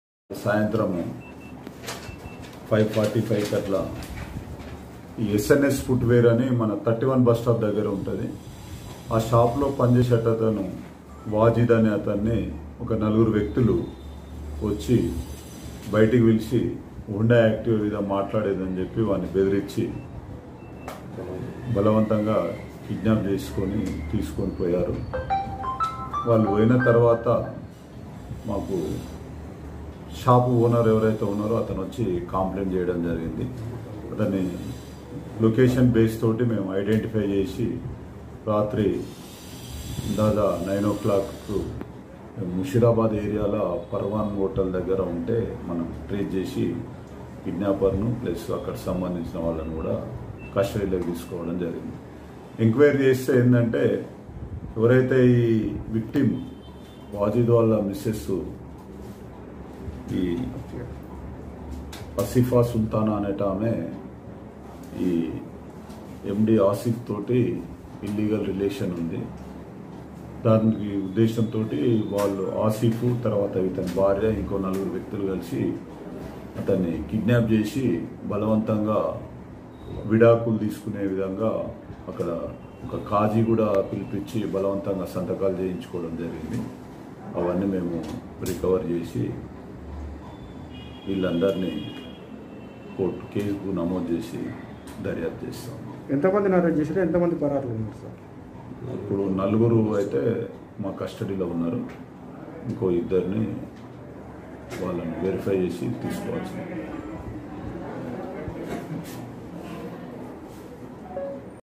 AASHIQ KE SATH MILKAR KHATOON NE KIYA APNE HI HUSBAND KO KIDNAP ! MARKET POLICE STATION LIMITS..
FOR DETAILS :-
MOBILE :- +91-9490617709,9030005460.
CALL EVERY TIMES NEWS SUB EDITOR, SHAIK MURTUZA
FOR DETAIL :-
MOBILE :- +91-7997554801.